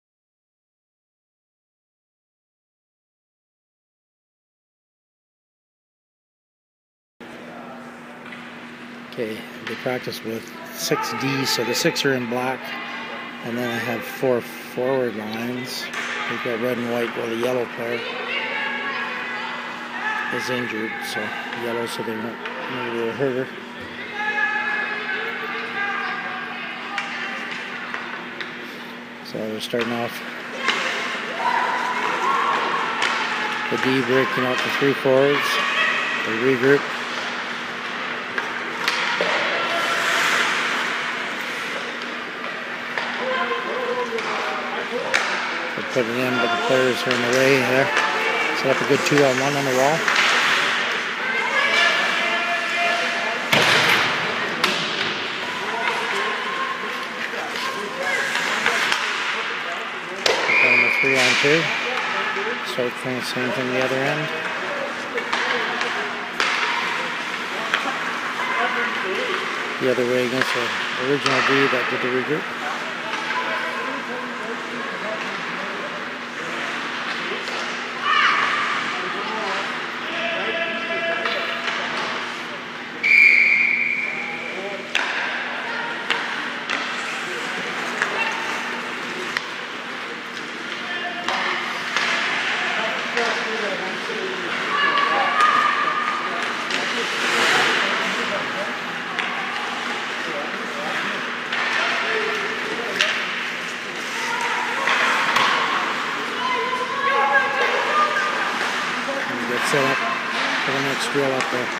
Ice Hockey Drills
C3 - 5-3 Regroup x 2 - Turn Back - Attack 3-2 - Pro W
Key Points:
F's give a target and face the puck. D's hinge and drive skate with the puck. Create a 2-1 on offense. Fight for rebounds. D control sticks in the slot and deflect attack.

Description:
1. Two D start at each blue line.
2. F1's regroup with D1's.
3. F1's regroup with D2's.
4. F1's turn back before crossing the blue line.
5. F1's attack 3-2 vs. D1's.
6. D3's replace D1's.
7. Repeat with F2's regrouping with D2's.